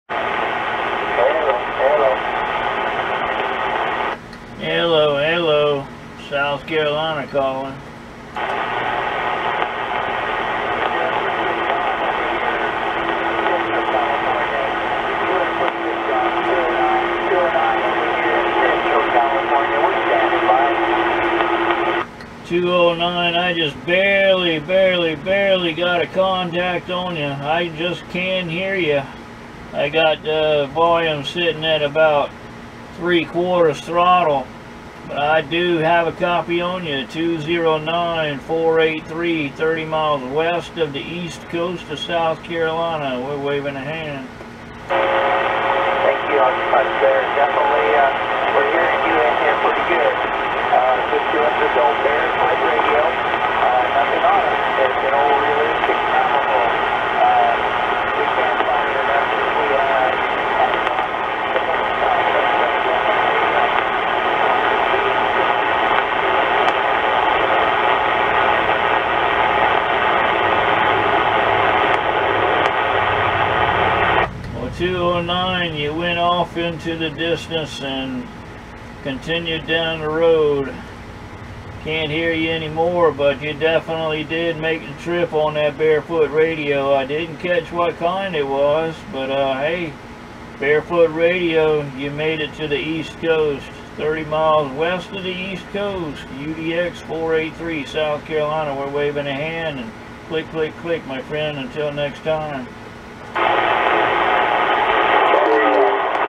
209, Central California, 2.7385 LSB 05/23/24
This is one of those that well, it wasn't a "Big Strapper" and it wasn't "Booming Audio", but I heard him call. I will answer anyone I can hear and will do my best to hear even with conditions as such. Most of us started with small stations like this and were really happy to get that return and especially a DX call. If I can hear you, I will return your call.